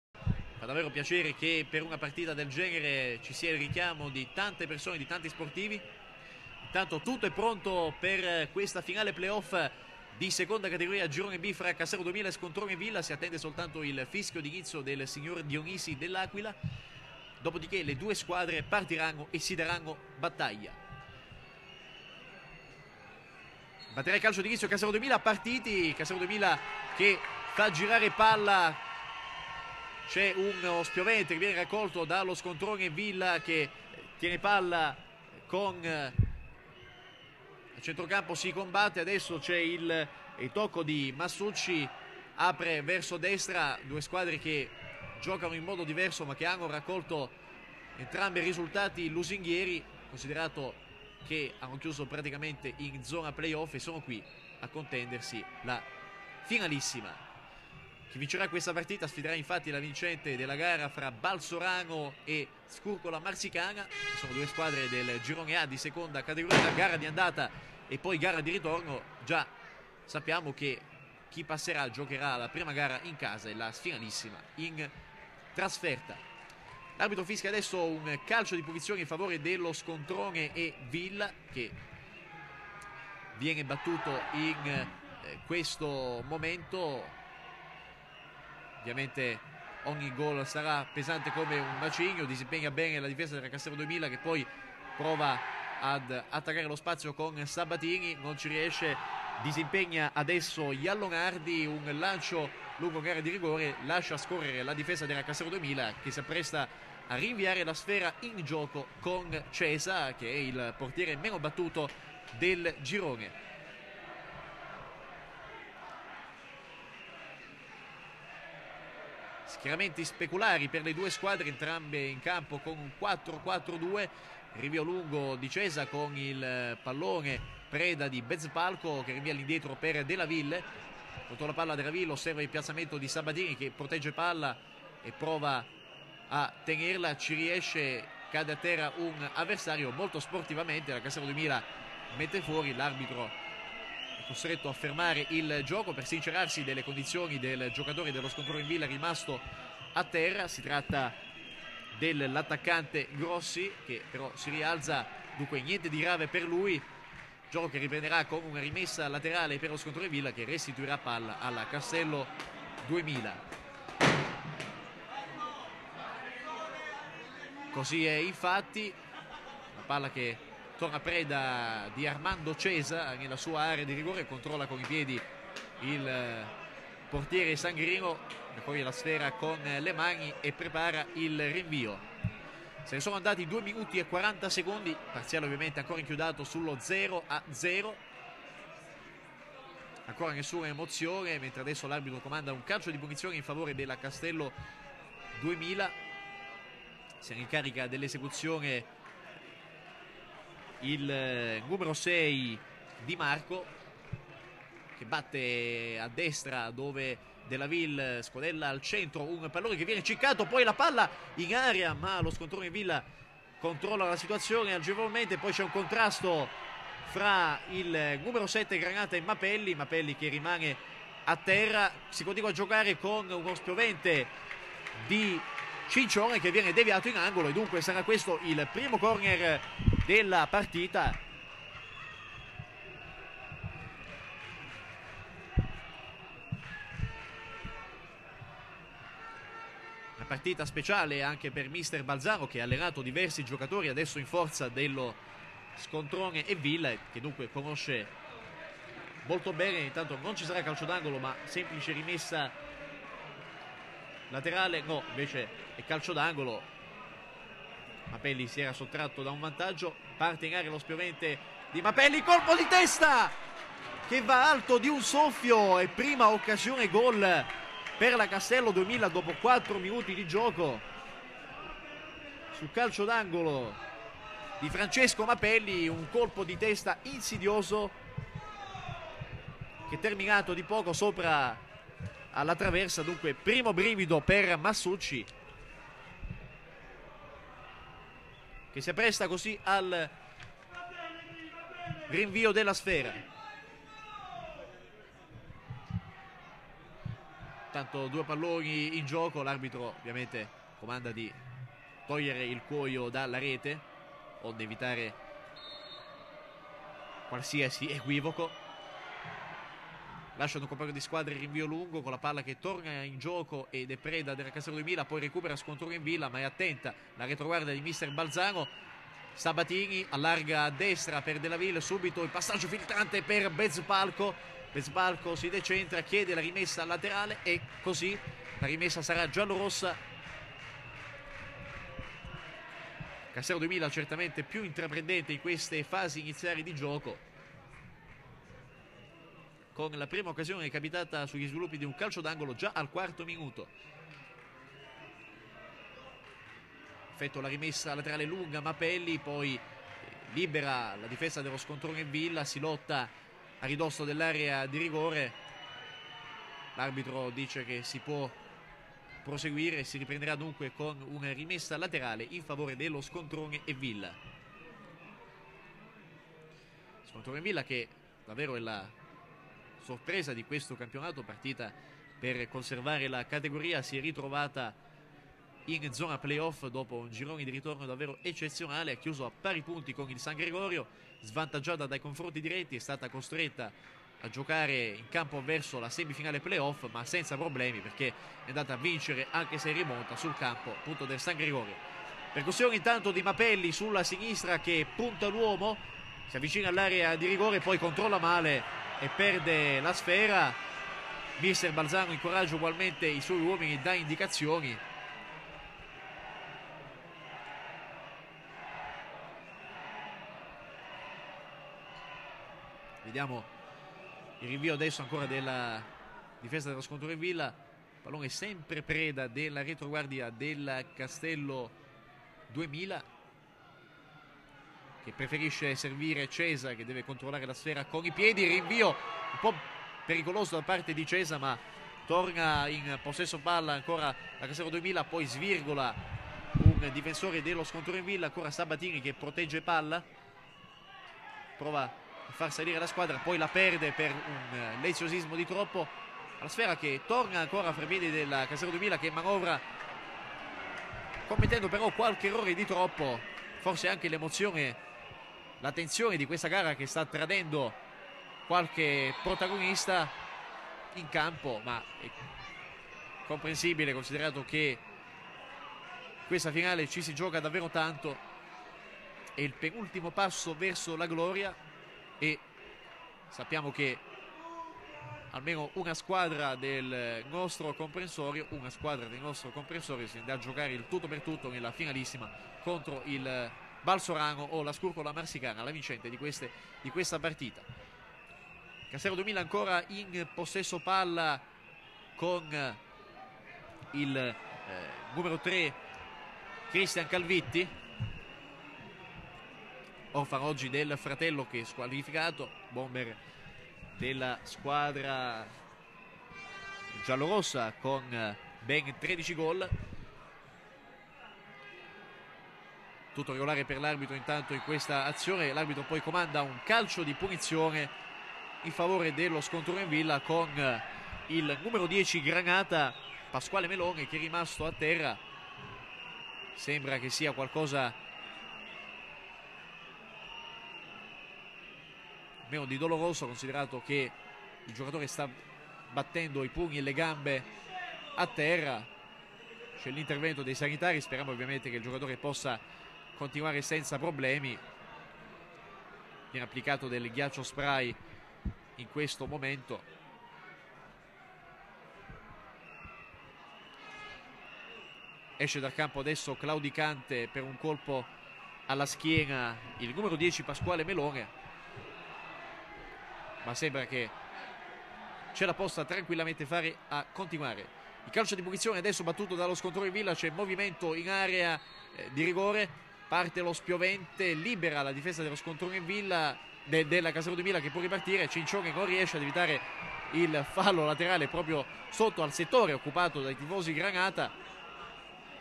Scontrone e Villa vs Castello 2000 - Play off - Parte 2
Grande match calcistico valido per i play-off di seconda categoria abruzzese tra la Castello 2000, formazione di Castel di Sangro, e lo Scontrone e Villa, formazione del vicinissimo comune di Scontrone.
Telecronaca di Marco Santopaolo, riprese di Sandro Riccio e Montaggio e grafica di Dario Riccio.

L'intera partita è stata divisa per necessità di YouTube in più parti. Questi i primi minuti della partita, a seguire c'è "Scontrone e Villa vs Castello 2000 - Play off - Parte 3" con i successivi istanti.